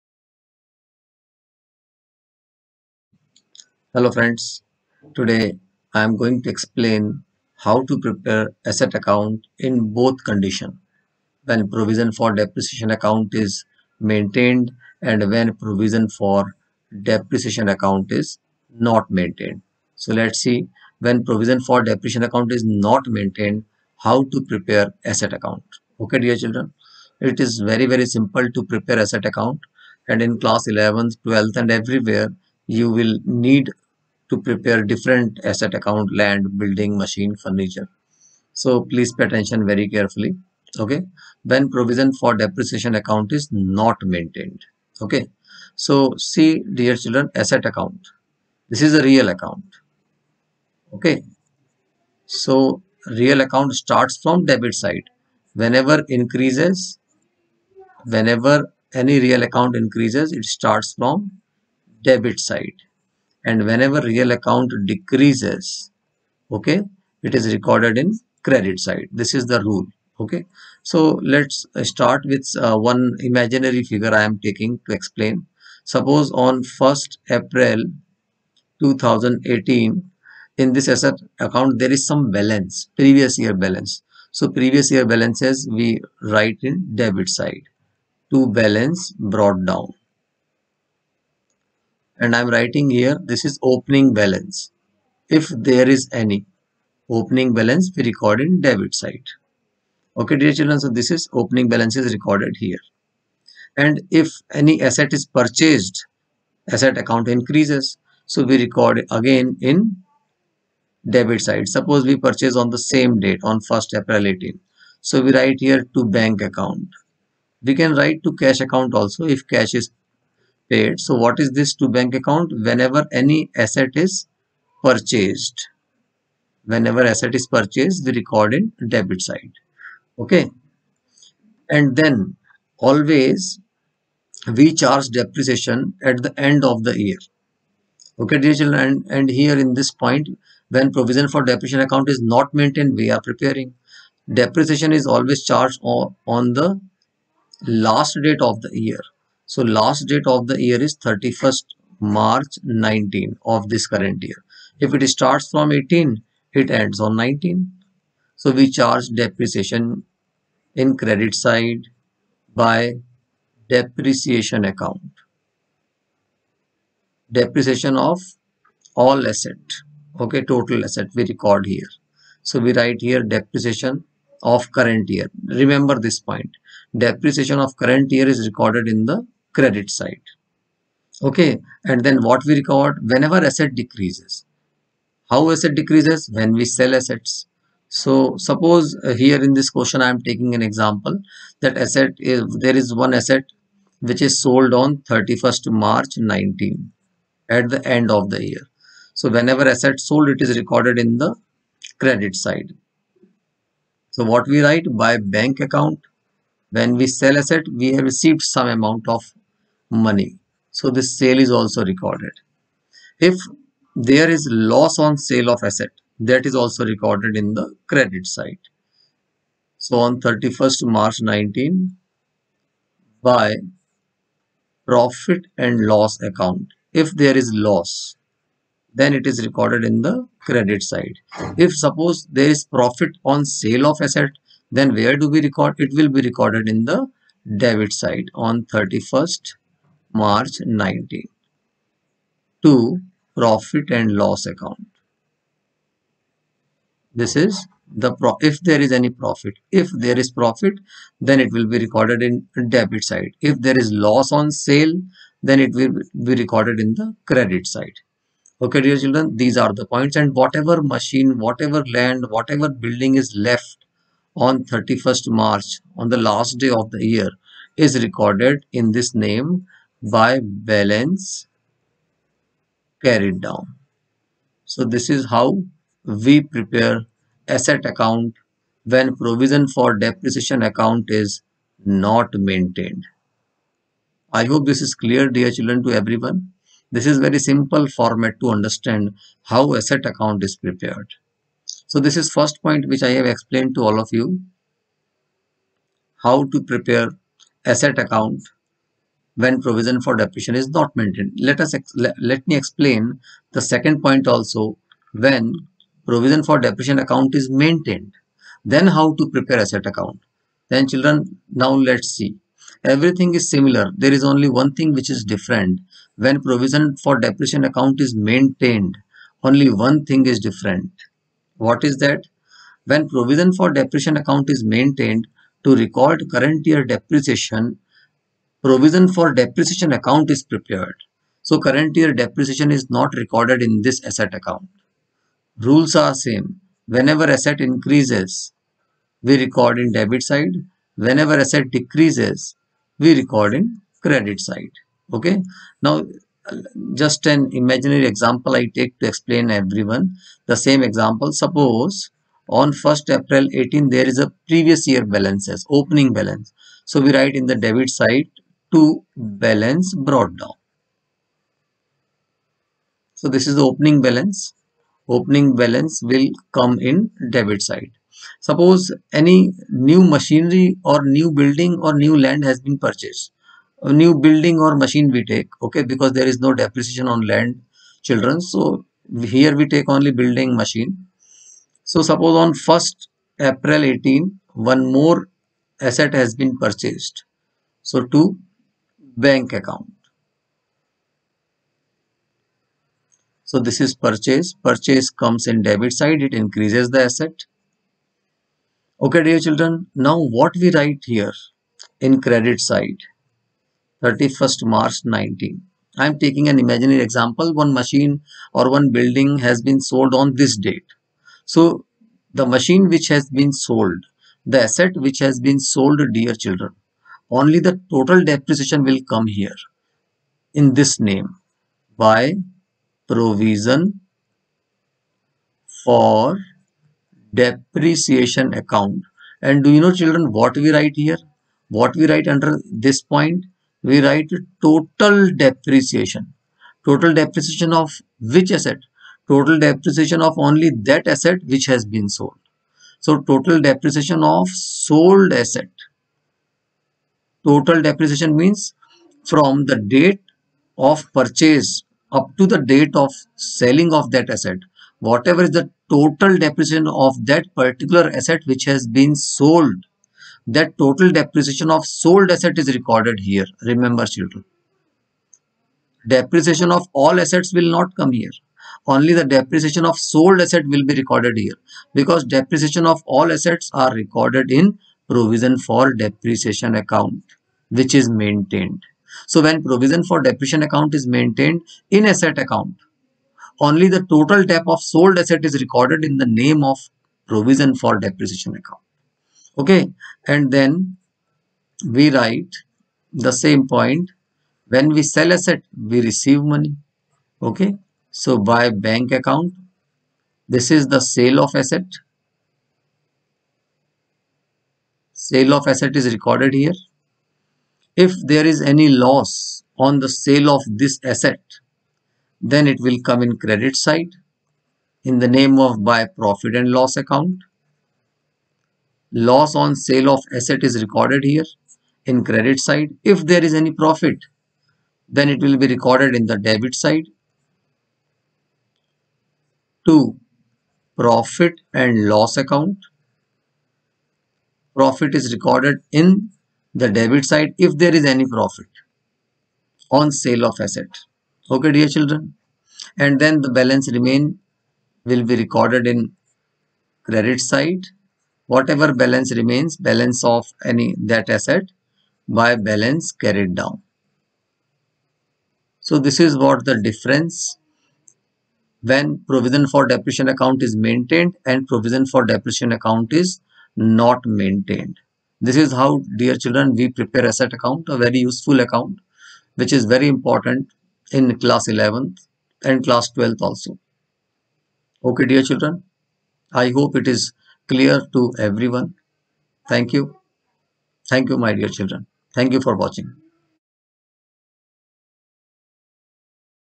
How to Prepare Fixed Asset Account | Class11th | Class 12th
How to Prepare Fixed Asset Account | Class11th | Class 12th | CBSE | Financial Accounting | How to prepare fixed asset account when provision for depreciation account is maintained or provision for depreciation account is not maintained | How to create or make fixed asset account |

JaiShriHari